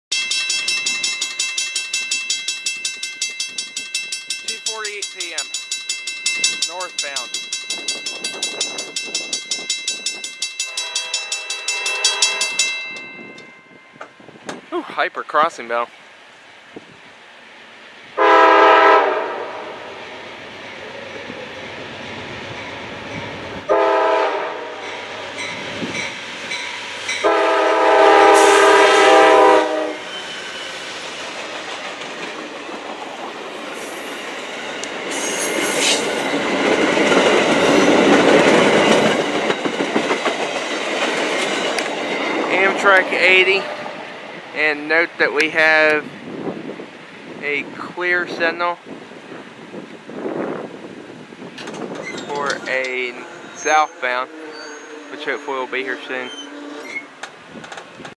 Amtrak P080
2:48 PM

I switched locations to Vaughn Road (North Ashland) to get some shot variety, where I would catch the next several trains. The first train was P080, the northbound Carolinian.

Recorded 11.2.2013